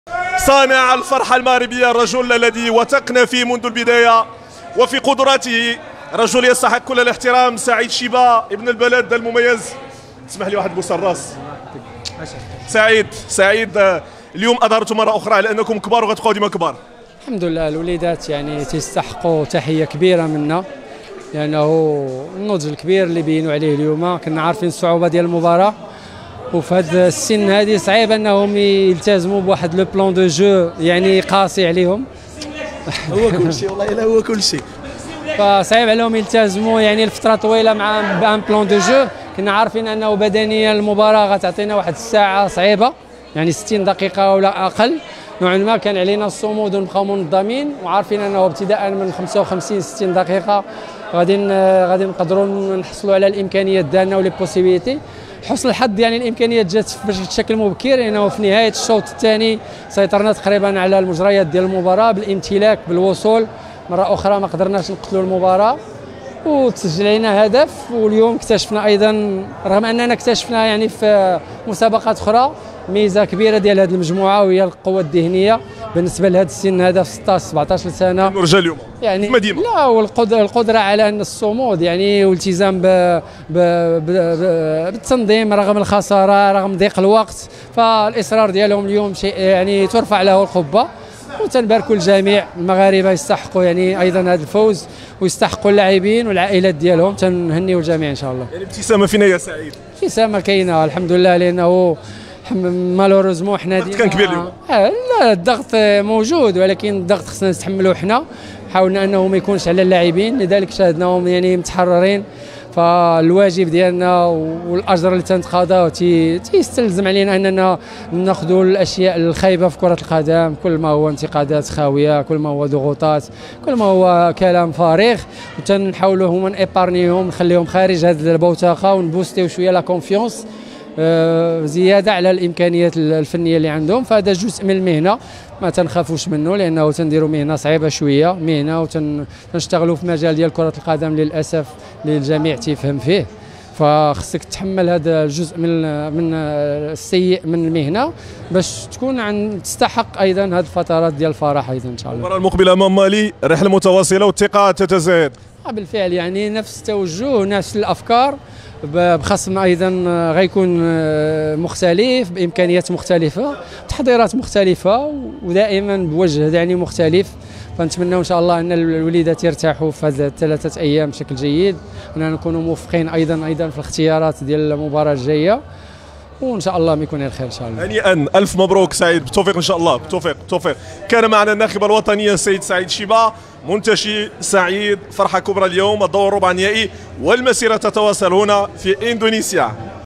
🎙️| سعيد شيبا مدرب المنتخب الوطني المغربي لأقل من 17 سنة "اللاعبون يستحقون منا كل الإشادة والتقدير "
‏Fédération Royale Marocaine de Football
‏ Équipe du Maroc FIFA U-17 World Cup